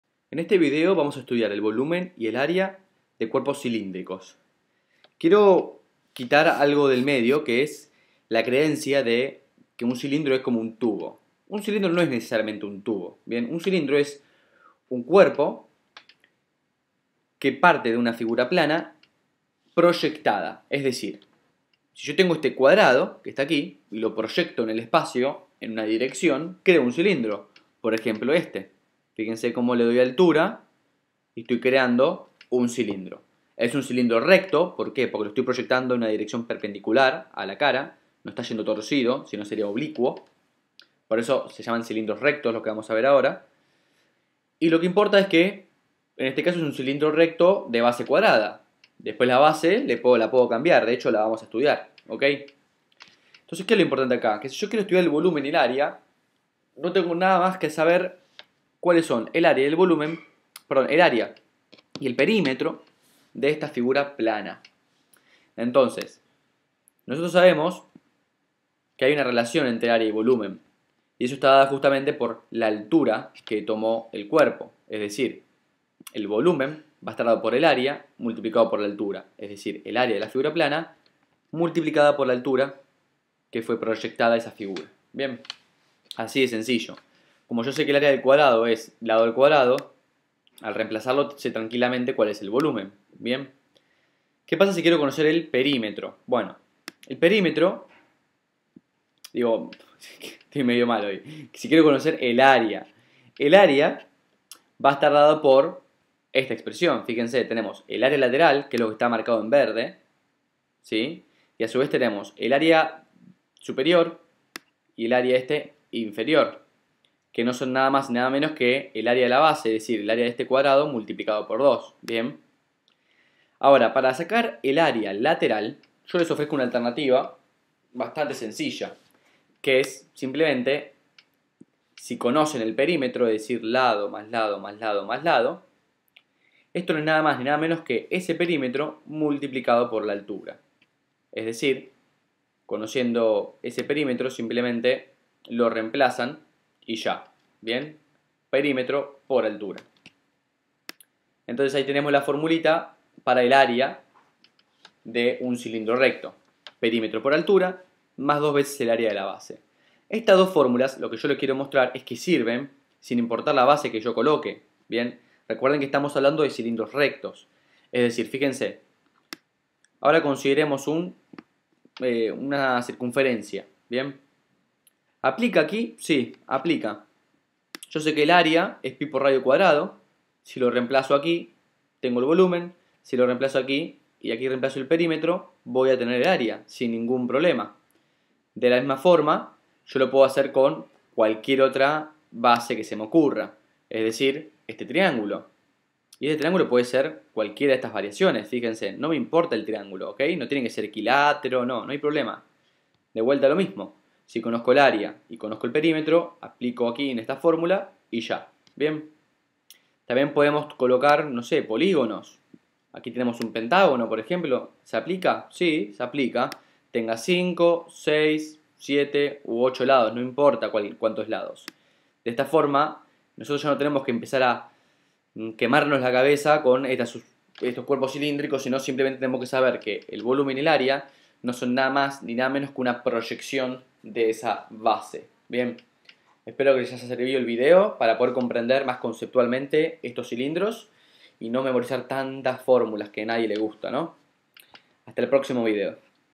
Cilindros rectos | DEDUCCION DE FORMULAS | GEOMETRIA
NO memorices, COMPRENDE 😉

Gracias por ser parte de este proyecto educativo gratuito.
Si te gustó el video, dale "me gusta", comenta y

Muchas gracias por ser parte.

Ing. Juan Ignacio Silva